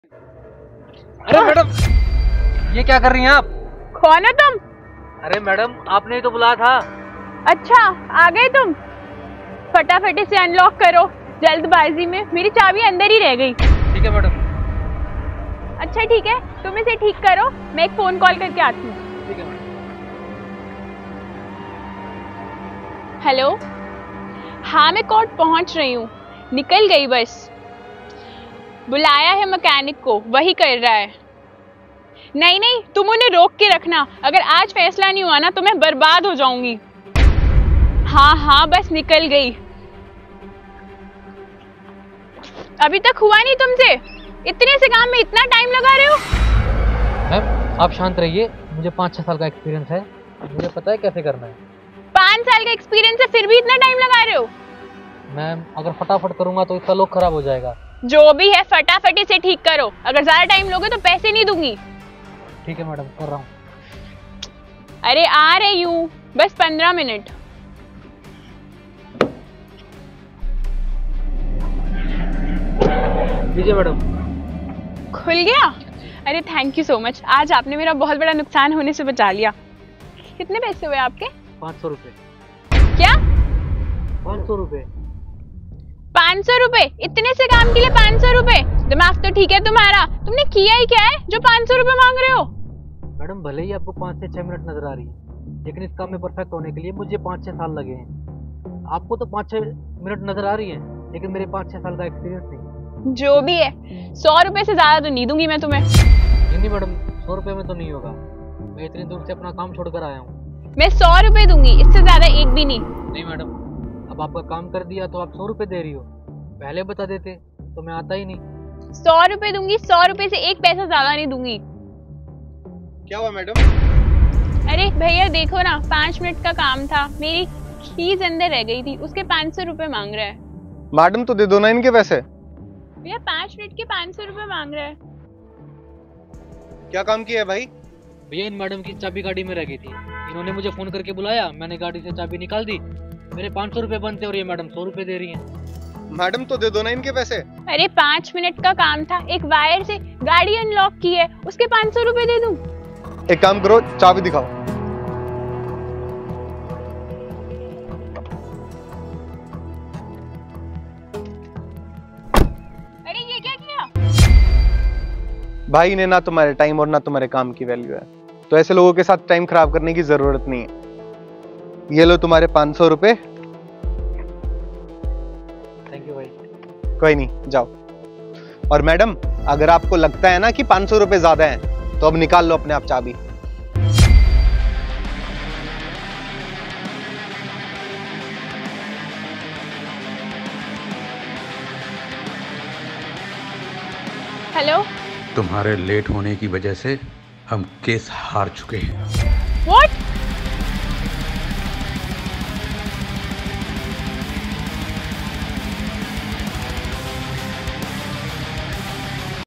Ladki ki car ki chaabi car ke andar reh gai | The Makers | Short Movie
Please comment and tell us, which part you like most and why?

DOP: @iishtom18
Editor: Shivraj Vidrohi
Cast: MohitACT17, Yasna, Vishal Gahlawat